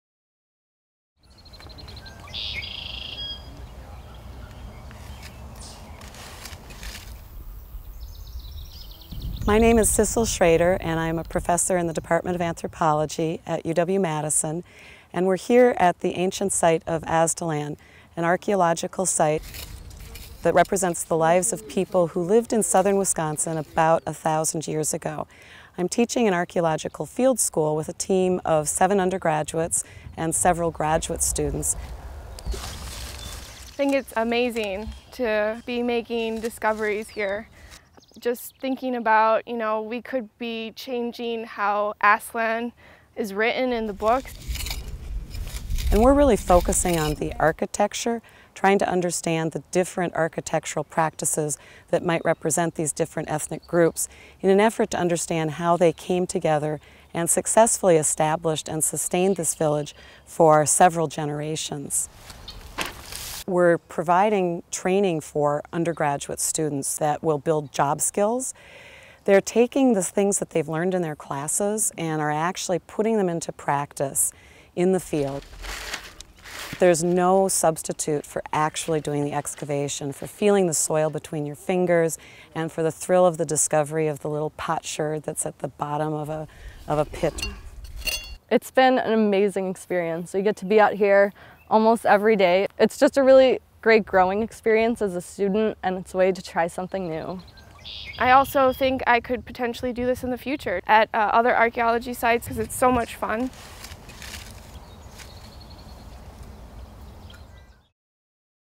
The Mystery of Aztalan Mounds
Students in the Field School for Archaeology at University of Wisconsin-Madison are working again at Aztalan, an ancient mound culture located between Madison and Milwaukee.  Professor Sissel Schroeder says the work is fleshing out our picture of the mound builders who worked here 1,000 years ago, and prepares students for careers in archaeology.